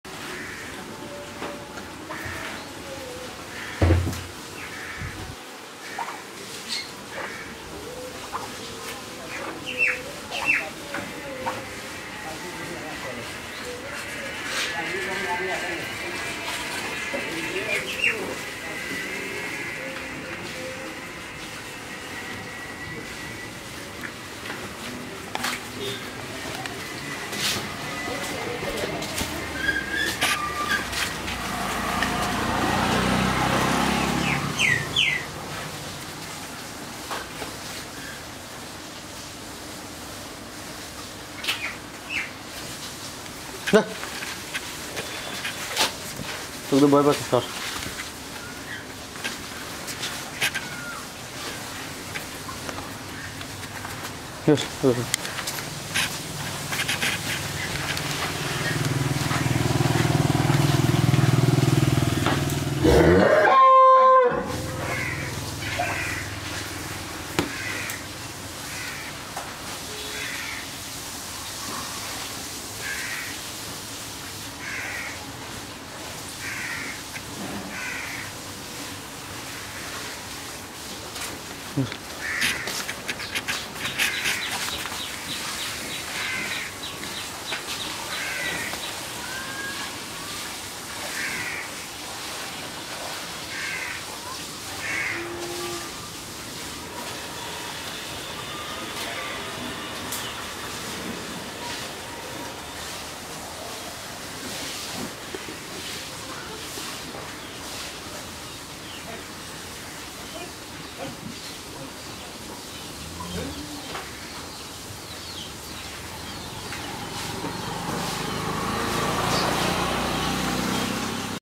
২০৭৩ নং // দ্বিতীয় বিয়ানের ফ্রিজিয়ান গাভীন বিক্রয় হবে। 01714843088// 01862268017
এটি একটি উন্নত মানের ফ্রিজিয়ান জাতের গাভী । গাভীটি এবার ২য় বারের মত বাচ্চা প্রসব করবে । গাভীটির  পেটে ৯ মাসের বাচ্চা আছে। গাভীটির পূর্বের দুুধের রেকর্ড অনুযায়ী এবারের দুধের পরিমান ১৮-২০ লিটার।
গুনে মানে অতুলনীয় গাভীটি একদম  রোগ বালাই মুক্ত।

দাম : অালোচনা সাপেক্ষে।